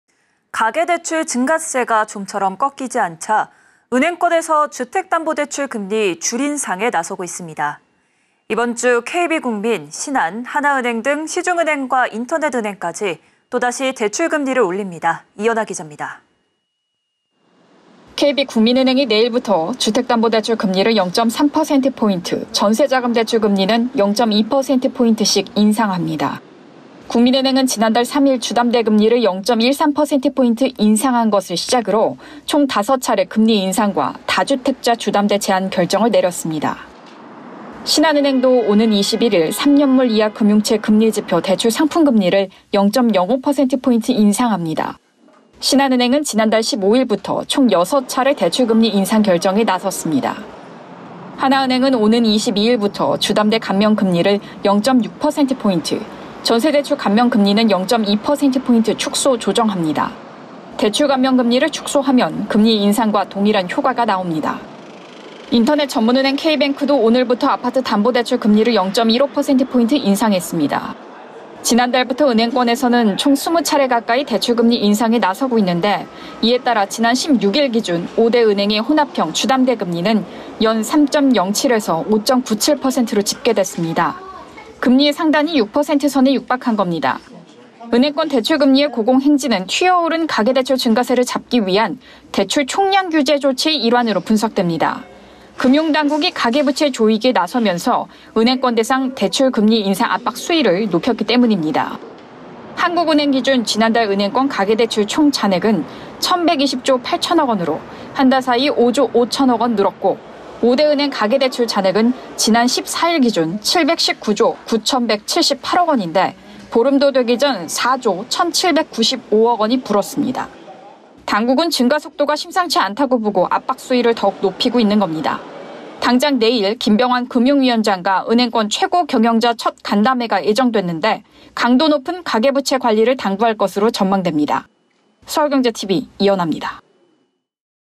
국민·신한·하나 주담대 금리 또 인상…가계부채 증가세 잡힐까
[앵커]
가계대출 증가세가 좀처럼 꺾이지 않자 은행권에서 주택담보대출 금리 줄인상에 나서고 있습니다. 이번 주 KB국민, 신한, 하나은행 등 시중은행과 인터넷은행까지 또다시 대출금리를 올립니다. 이연아 기자입니다.

[기자]
KB국민은행이 내일(20일)부터 주택담보대출 금리를 0.3%p, 전세자금대출 금리는 0.2%p씩 인상합니다.

국민은행은 지난달 3일 주담대 금리를 0.13%p 인상한 것을 시작으로 총 다섯 차례 금리 인상과 다주택자 주담대 제한 결정을 내렸습니다.

신한은행도 오는 21일 3년물 이하 금융채 금리 지표 대출상품 금리를 0.05%p 인상합니다.
신한은행은 지난달 15일부터 총 여섯 차례 대출금리 인상 결정에 나섰습니다.

하나은행은 오는 22일부터 주담대 감면금리를 0.6%p, 전세대출 감면금리는 0.2%p 축소 조정합니다.
대출 감면금리를 축소하면, 금리 인상과 동일한 효과가 나옵니다.

인터넷전문은행 케이뱅크도 오늘(19일)부터 아파트담보대출 금리를 0.15%p 인상했습니다.

지난달부터 은행권에서는 총 20차례 가까이 대출금리 인상에 나서고 있는데, 이에 따라 지난 16일 기준 5대 은행의 혼합형 주택담보대출 금리는 연 3.07~5.97%로 집계됐습니다. 금리 상단이 6%선에 육박한 겁니다.

은행권 대출금리의 고공행진은 튀어오른 가계대출 증가세를 잡기 위한 대출 총량 규제 조치의 일환으로 분석됩니다. 금융당국이 가계부채 조이기에 나서면서, 은행권 대상 대출금리 인상 압박 수위를 높였기 때문입니다.

한국은행 기준 지난달 은행권 가계대출 총 잔액은 1,120조8,000억원으로 한 달 사이 5조5,000억원 늘었고, 5대 은행 가계대출 잔액은 지난 14일 기준 719조9,178억 원인데, 보름도 되기 전 4조1,795억원이 불었습니다.
당국은 증가속도가 심상치 않다고 보고 압박 수위를 더욱 높이고 있는 겁니다.
당장, 내일(20일) 김병환 금융위원장과 은행권 최고경영자 첫 간담회가 예정됐는데, 강도 높은 가계부채 관리를 당부할 것으로 전망됩니다.